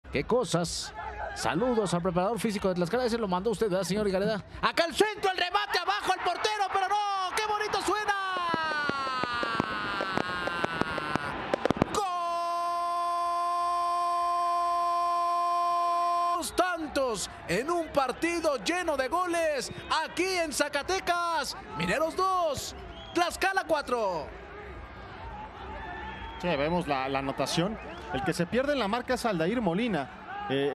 Gol de E. Monreal| Mineros 2-4 Tlaxcala | Jornada 14- Grita X La Paz - Liga BBVA Expansión MX
¡G⚽⚽⚽⚽L de Mineros!⚽🔥

¡Acortan distancia!

Con este cabezazo, Ernesto Monreal acerca a la escuadra zacatecana al poner el 2-4 en el marcador.